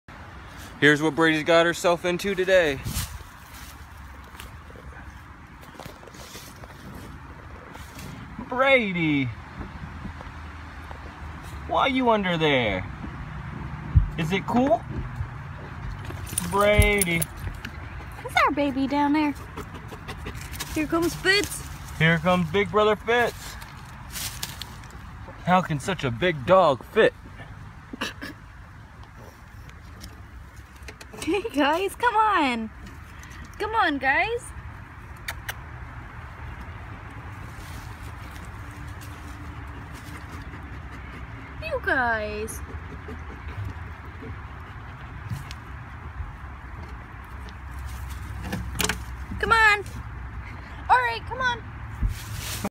Big dogs in tight spots
Brady and Fitz fit themselves in a tight spot. Daww